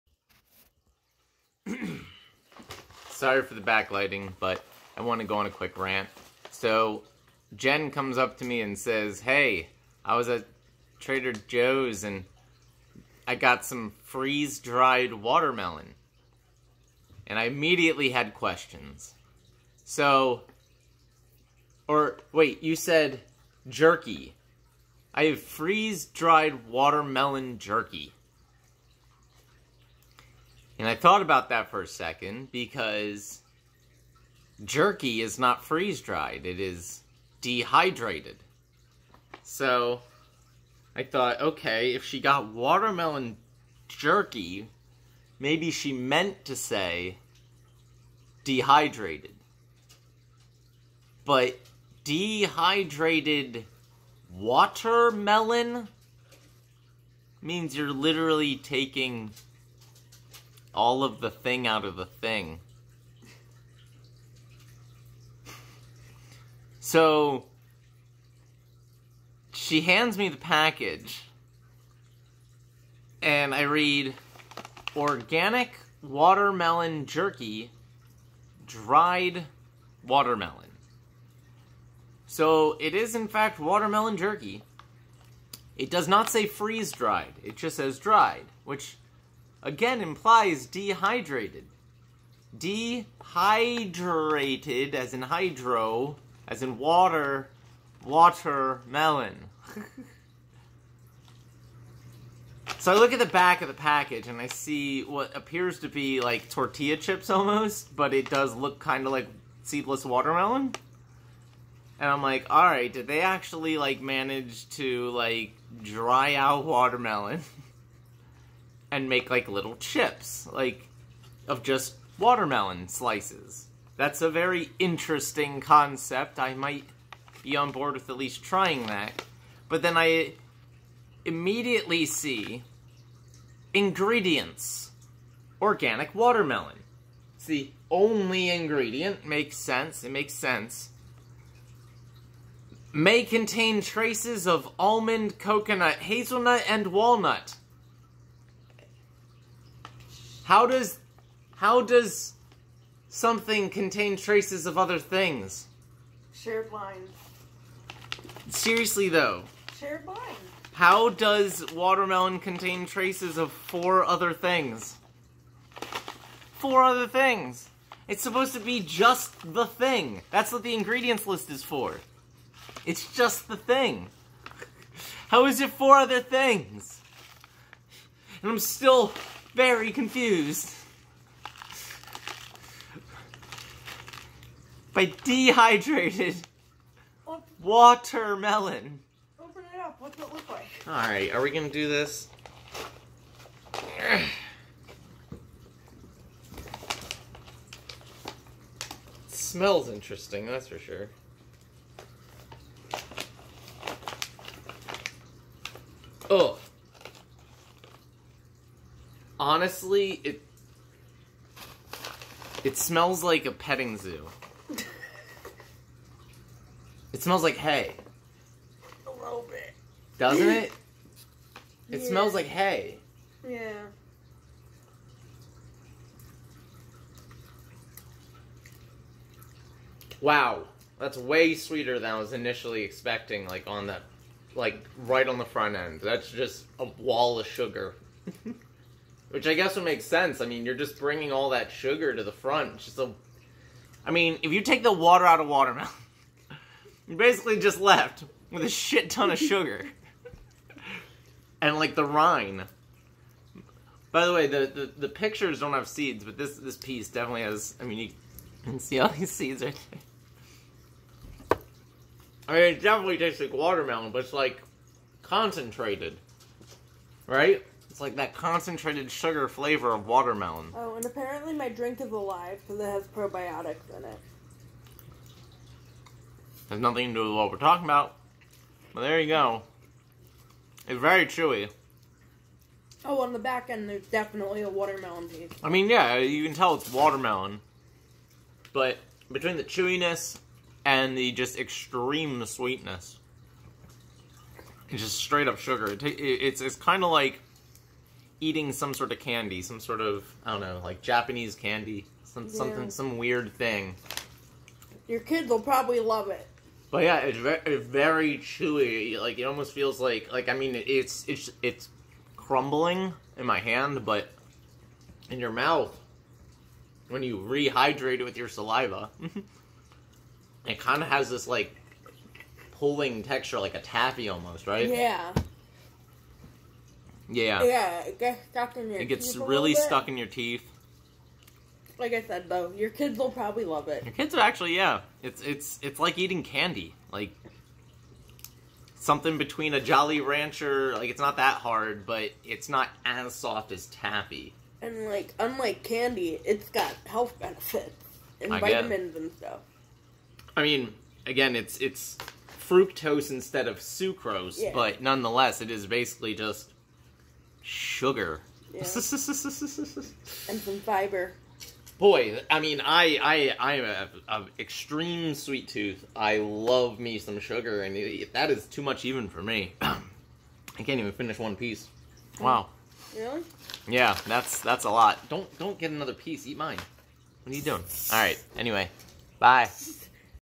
Trying deHYDRATed WATERmelon
I try a product called "watermelon jerky" which is somehow dehydrated watermelon pieces. It's basically candy 🍬